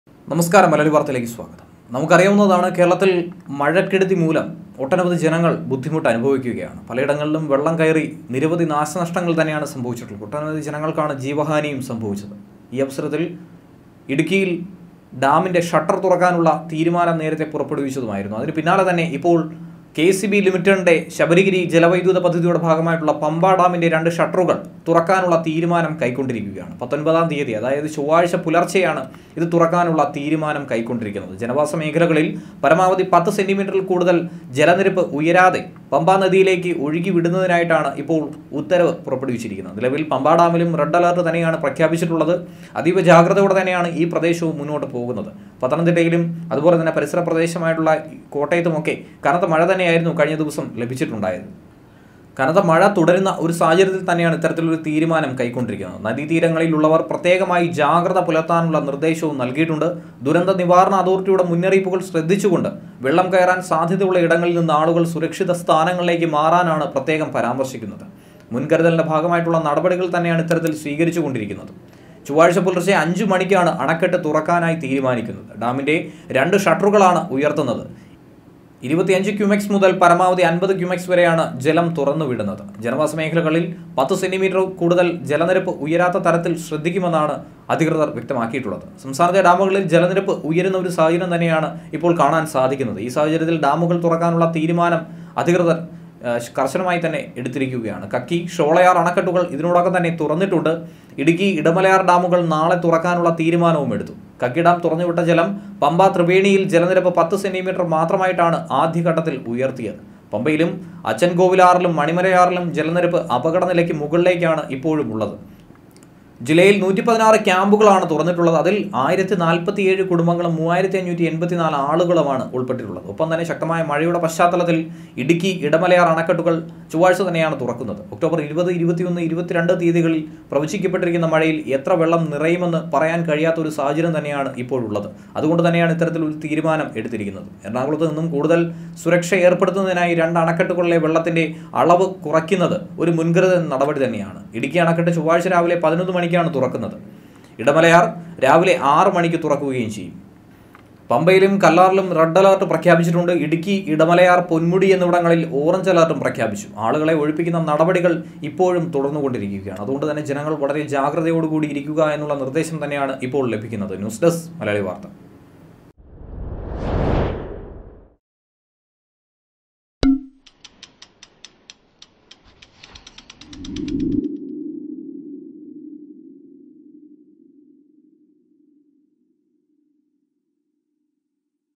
Sabari ജനങ്ങൾ ശ്രദ്ധിക്കേണ്ടത് ഇതാണ്...
ചൊവ്വാഴ്ച പുലർച്ചെ 5 മണിക്കാണ് അണക്കെട്ട് തുറക്കാൻ തീരുമാനിച്ചിരിക്കുന്നത്. ഡാമിന്റെ രണ്ട് ഷട്ടറുകളാണ് ഉയർത്തുന്നത്.